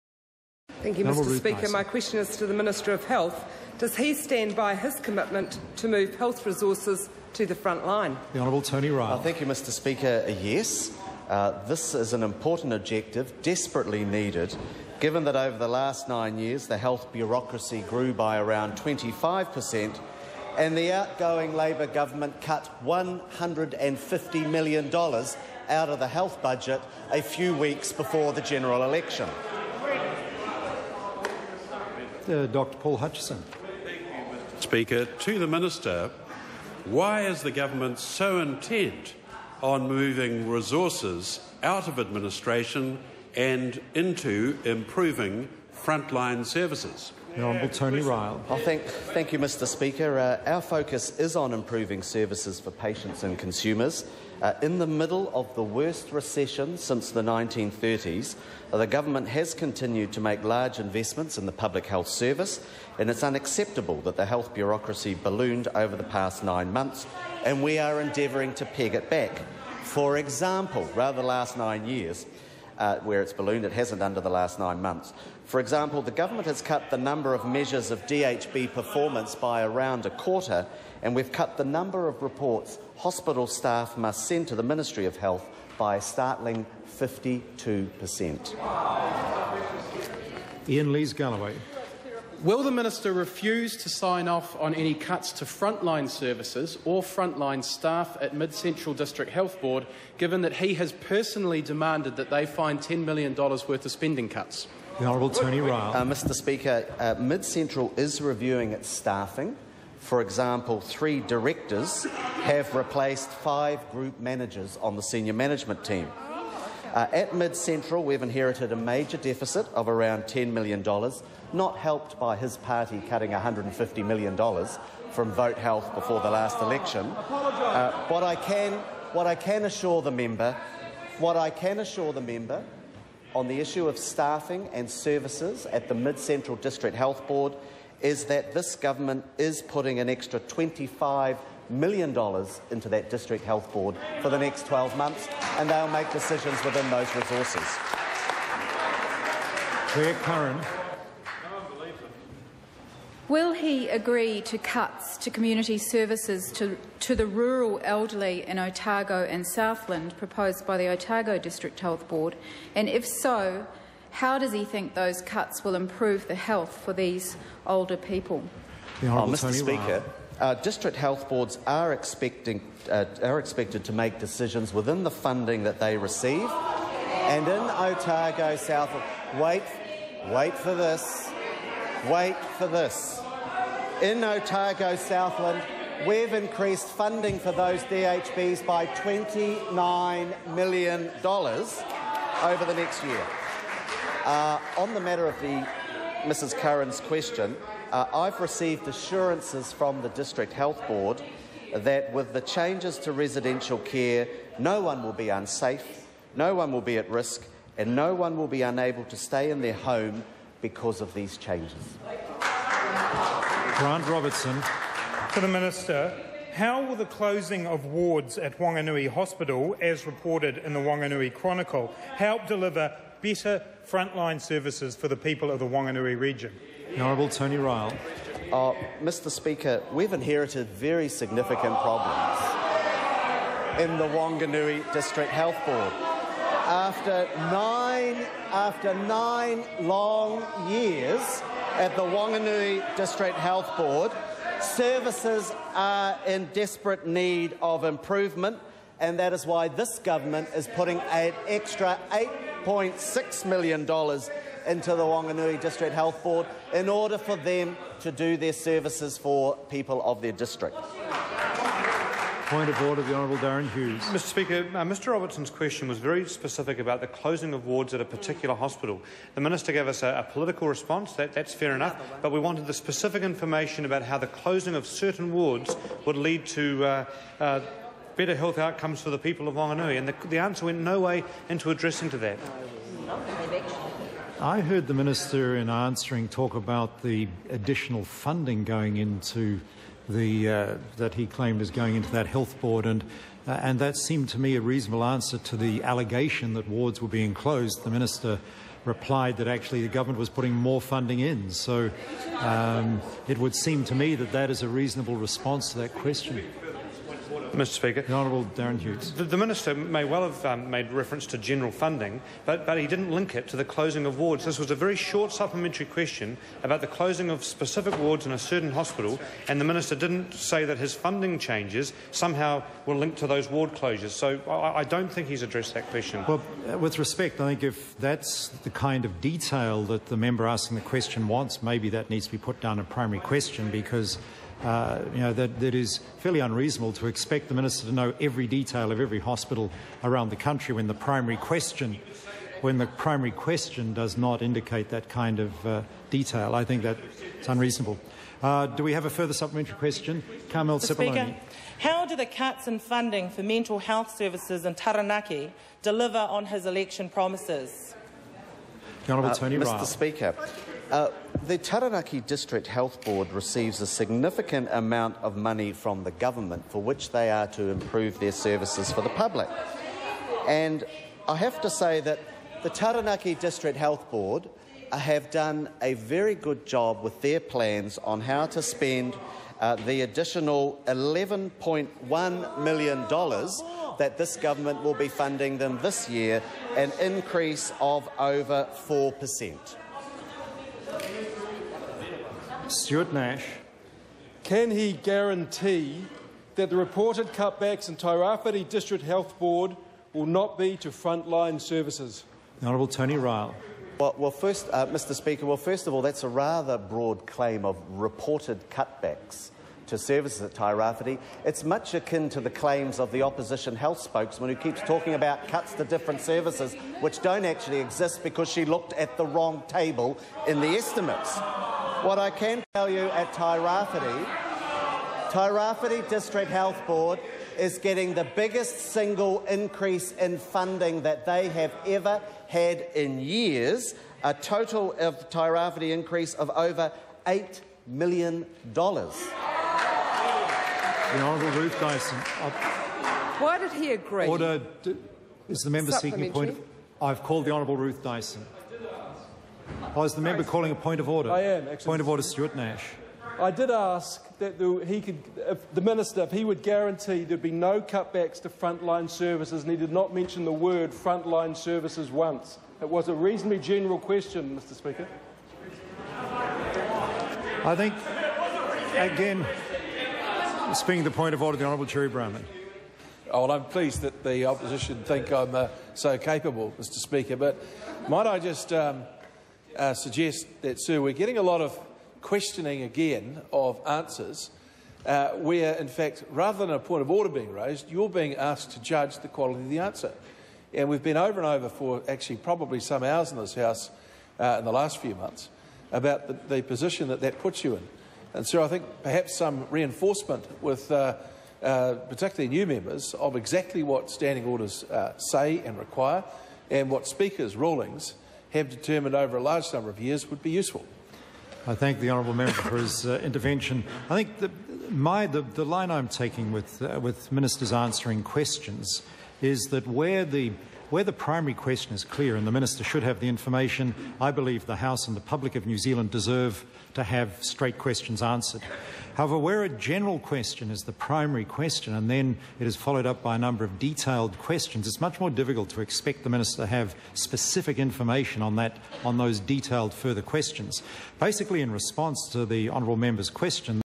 August05 Q1 part 1
1. Hon RUTH DYSON (Labour—Port Hills) to the Minister of Health: Does he stand by his commitment to move health resources to the front line?

Hon TONY RYALL (Minister of Health) : Yes. This is an important objective that is desperately needed, given that over the last 9 years the health bureaucracy grew by around 25 percent, and the outgoing Labour Government cut $150 million from the health budget a few weeks before the general election.

Dr Paul Hutchison: Why is the Government so intent on moving resources out of administration and into improving front-line services?

Hon TONY RYALL: Our focus is on improving services for patients and consumers. In the middle of the worst recession since the 1930s, the Government has continued to make large investments in the public health service, and it is unacceptable that the health bureaucracy has ballooned over the past 9 years. We are endeavouring to peg it back. For example, the Government has cut the number of measures of district health board performance by around a quarter, and we have cut the number of reports that hospital staff must send to the Ministry of Health by a startling 52 percent.

Iain Lees-Galloway: Will the Minister refuse to sign off on any cuts to front-line services or front-line staff at MidCentral District Health Board, given that he has personally demanded that it finds $10 million worth of spending cuts?

Hon TONY RYALL: The MidCentral District Health Board is reviewing its staffing. For example, three directors have replaced five group managers on the senior management team. At the MidCentral District Health Board we have inherited a major deficit of around $10 million, which is not helped by that members party cutting $150 million from Vote Health before the last election. But I assure the member that on the issue of staffing and services at the MidCentral District Health Board, this Government is putting an extra $25 million into that district health board for the next 12 months, and it will make the decisions with regard to those resources.